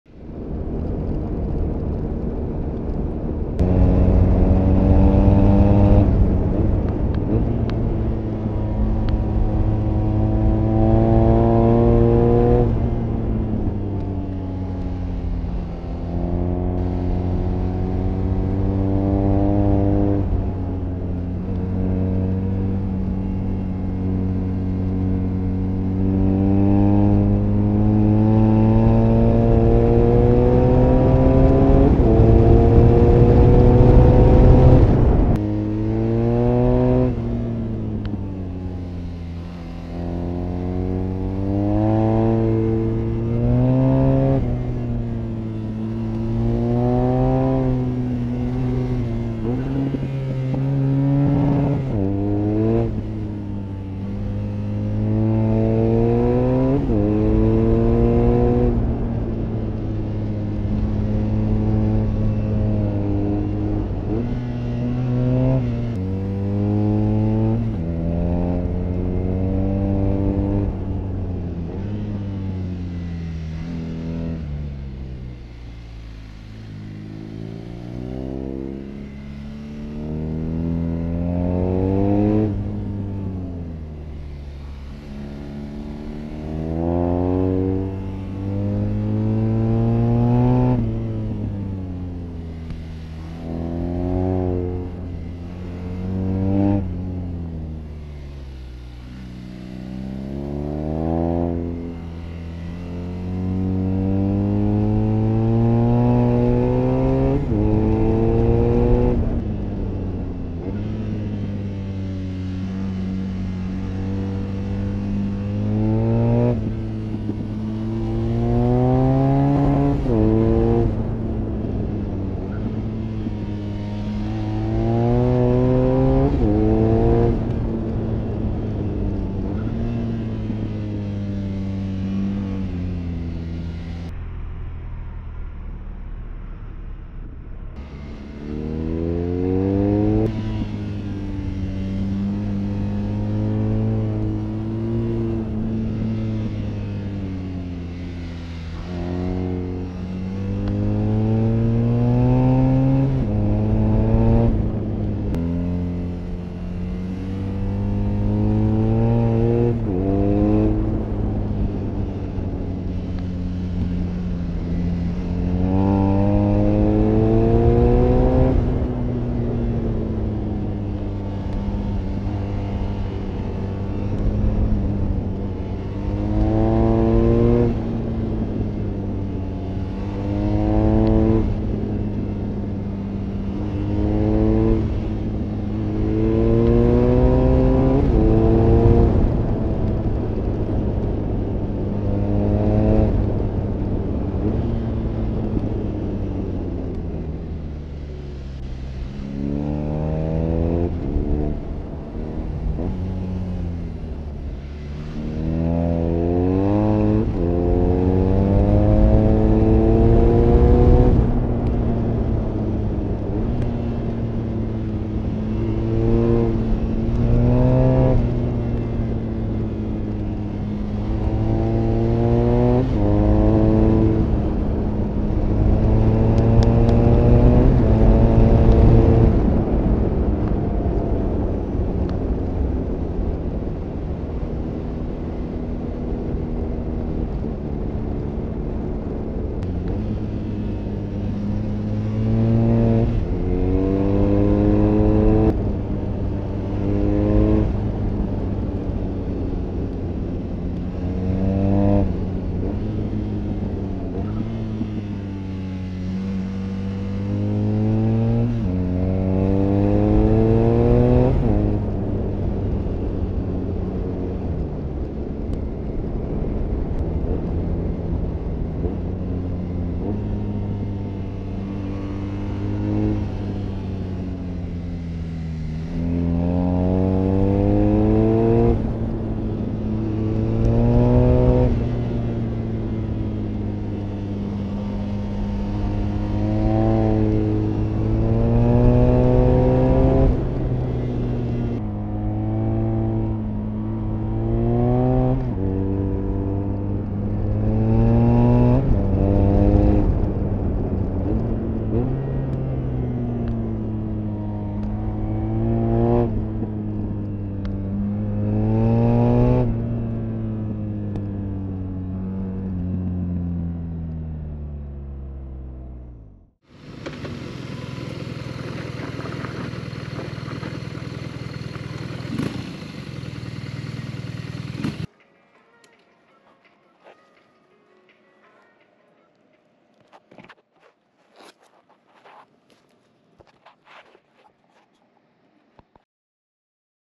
POV- BMW R nineT *100 Years edition* [RAW SOUND]
Mic could still be better, second time out on the bike. More clips soon!